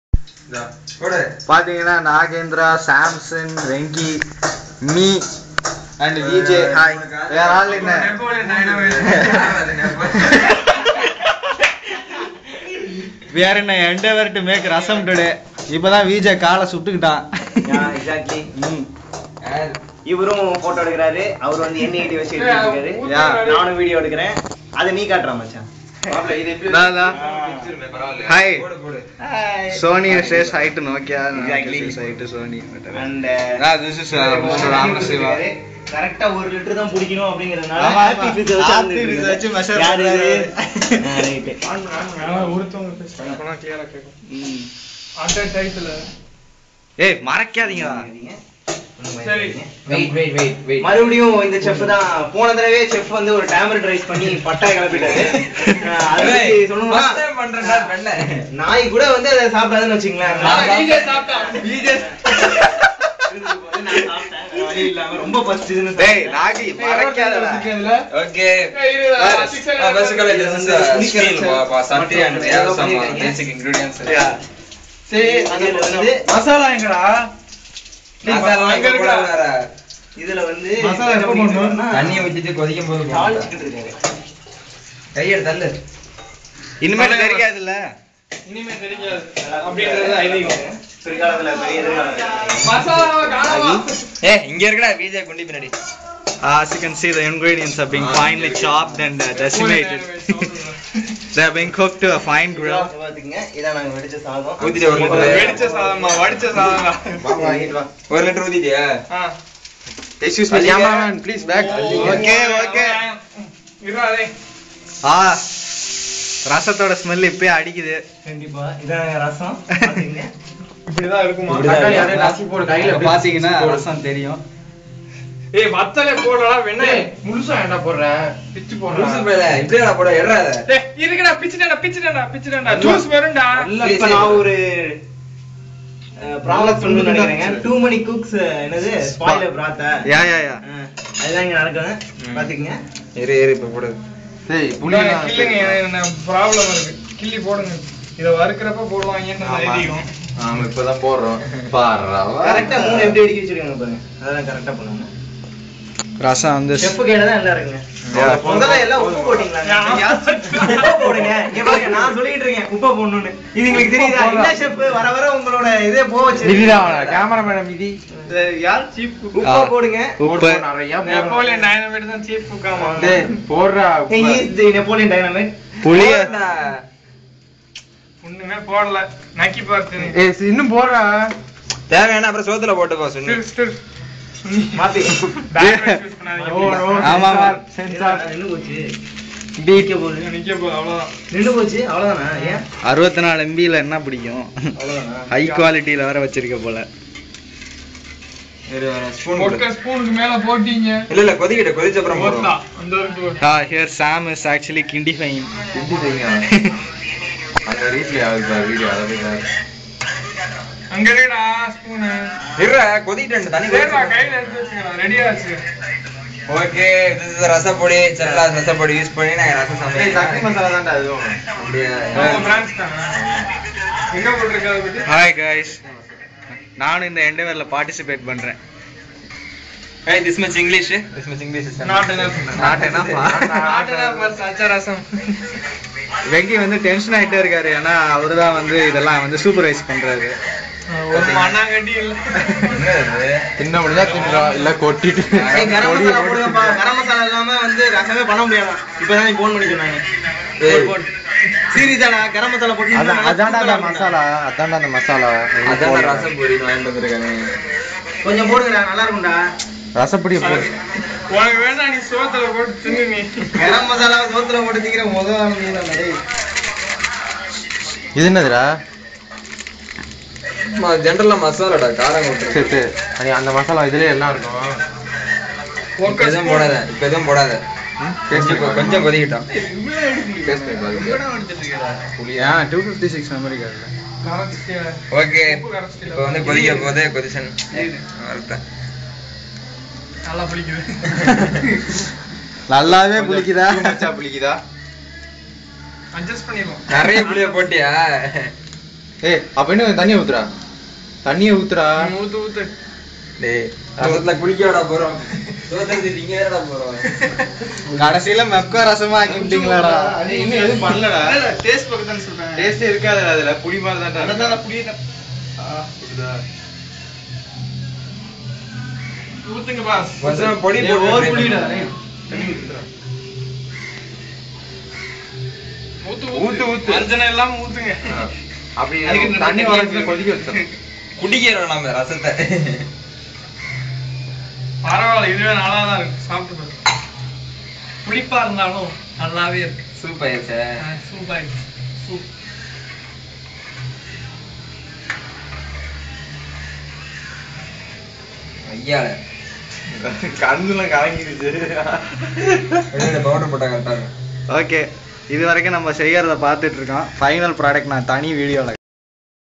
Making Rasam as dayscholars (MEPCO) - Part 1
We as dayscholars, venki, vj, kamal, sam, nagendra and ramana siva had a swell lotta fun makin rasam!